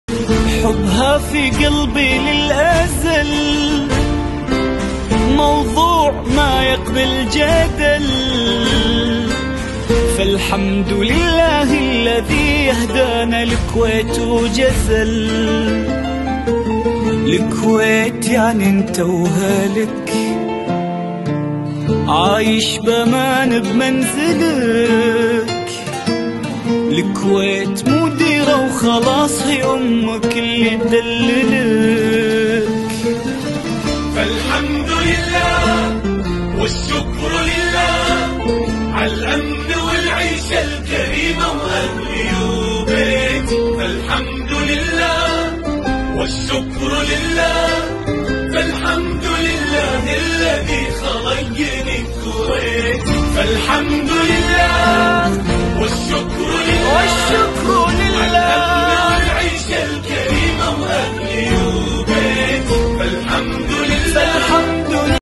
من حملة ابشري_ياكويت التي اطلقتها قبيلتي سبيع و السهول في دور الرعاية
من حملة
التي اطلقتها قبيلتي سبيع و السهول
توزيع وجبات افطار صائم في دور الرعايه
في ثاني أيام شهر رمضان المبارك